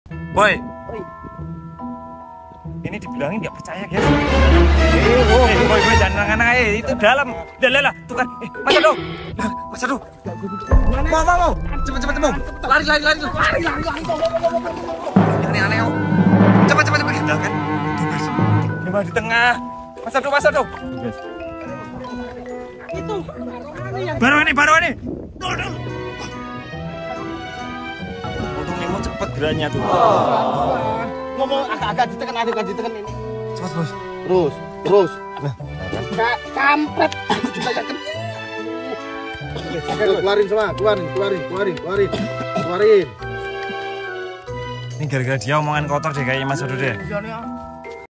Mas Boy GWSM nyaris tenggelam waktu mandiin barongan Ical 😭😱
Sumber: Channel Garuda Wisnu Satria Muda
Episode: HAMPIR AJA BOY GAK SELAMAT !! Niatnya mandiin barongan malah begini jadinya